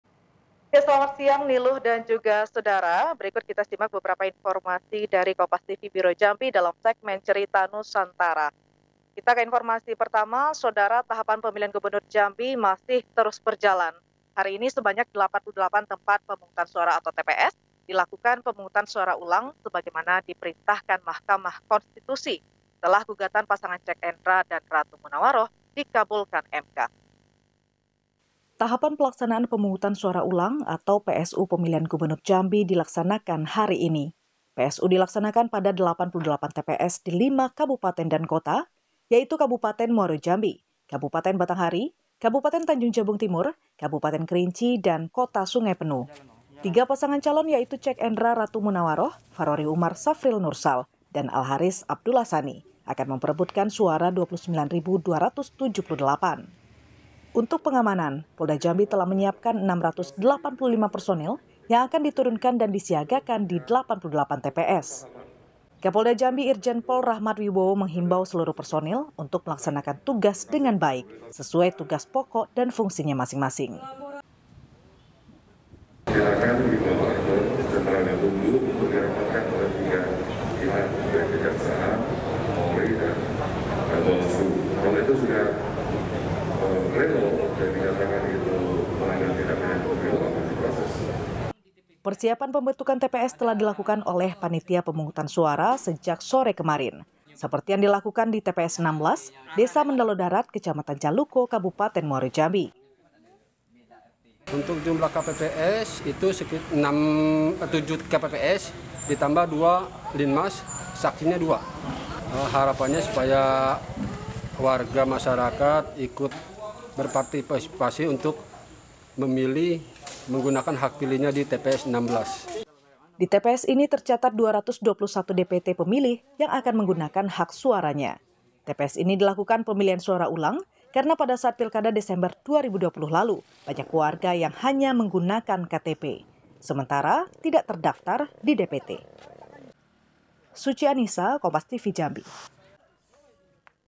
88 TPS Gelar Pemilihan Suara Ulang Gubernur Jambi
JAMBI, KOMPAS.TV - Tahapan pemilihan Gubernur Jambi masih terus berjalan, hari ini (27/05/2021) sebanyak 88 TPS melakukan pemungutan suara ulang sebagaimana diperintahkan Mahkamah Konstitusi, setelah gugatan pasangan Cek Endra dan Ratu Munawarah dikabulkan MK.

Tahapan pelaksanaan Pemungutan Suara Ulang atau PSU pemilihan Gubernur Jambi dilaksanakan hari ini.

PSU dilaksanakan pada 88 TPS di 5 kabupaten dan kota, yaitu Kabupaten Muaro Jambi, Kabupaten Batanghari, Kabupaten Tanjung Jabung Timur, Kabupaten Kerinci dan Kota Sungai Penuh.

Tigas pasangan calon yaitu, Cek Endra-Ratu Munawaroh, Fachrori Umar-Syafril Nursal, dan Al Haris - Abdullah Sani akan memperebutkan suara 29.278.

Untuk pengamanan, Polda Jambi telah menyiapkan 685 personel yang akan diturunkan dan disiaga kan di 88 TPS.

Kapolda Jambi Irjen Pol Rachmad Wibowo menghimbau seluruh personel untuk melaksanakan tugas dengan baik sesuai tugas pokok dan fungsinya masing-masing.

Persiapan pembentukan TPS telah dilakukan oleh panitia pemungutan suara sejak sore kemarin.

Seperti yang dilakukan di TPS Desa Mendalo Darat, Kecamatan Jaluko, Kabupaten Muaro Jambi.

Di TPS ini tercatat 221 DPT pemilih yang akan menggunakan hak suaranya.

TPS ini dilakukan pemilihan suara ulang karena pada saat Pilkada Desember 2020 lalu banyak warga yang hanya menggunakan KTP sementara tidak terdaftar di DPT.